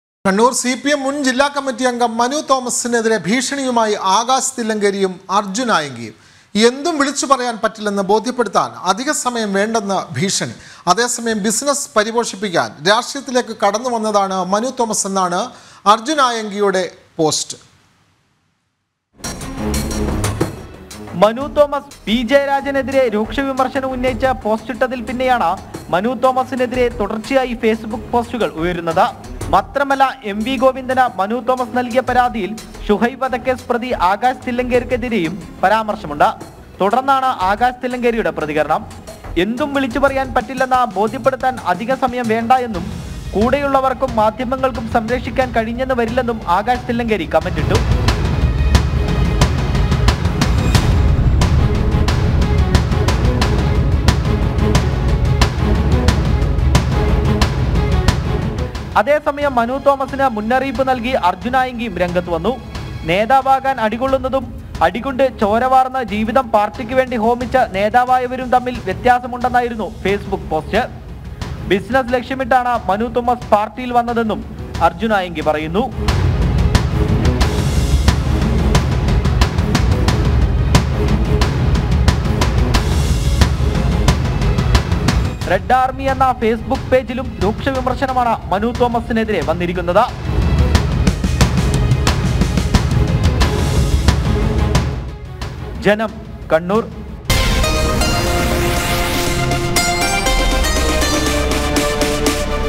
CPM മുൻ ജില്ലാ കമ്മിറ്റി അംഗം മനു തോമസിനെതിരെ ഭീഷണിയുമായി ആകാശ് തില്ലങ്കേരിയും അർജുൻ ആയങ്കിയും
കണ്ണൂർ CPM മുൻ ജില്ലാ കമ്മിറ്റി അംഗം മനു തോമസിനെതിരെ ഭീഷണിയുമായി ആകാശ് തില്ലങ്കേരിയും അർജുൻ ആയങ്കിയും. എന്തും വിളിച്ചു പറയാൻ പറ്റില്ലെന്ന് ബോധ്യപ്പെടുത്താൻ അധികസമയം വേണ്ടെന്ന് ഭീഷണി. അതേസമയം, ബിസിനസ് പരിപോഷിക്കാൻ രാഷ്ട്രീയത്തിലേക്ക് കടന്നുവന്നതാണ് മനു തോമസ് എന്നാണ് അർജുൻ ആയങ്കിയുടെ പോസ്റ്‍റ്.